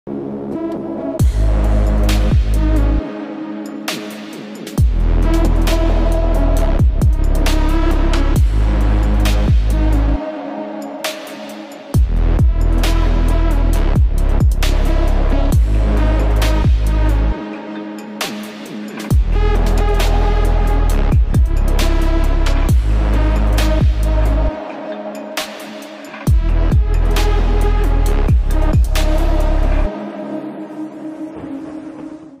Best Model of Azerbaijan 2022 - Press Conference
National Director and Organizer of Best Model Azerbaijan - Elkhan Pashayev